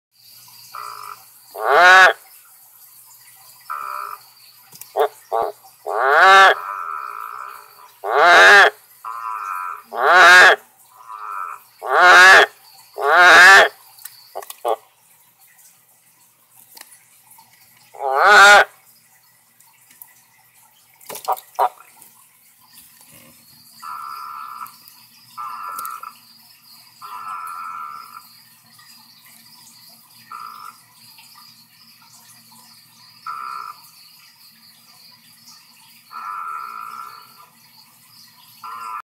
Male Amazon milk frog getting his flirt on
Male Amazon milk calling and trying to convince a female that the filter box is a good spot to lay her eggs.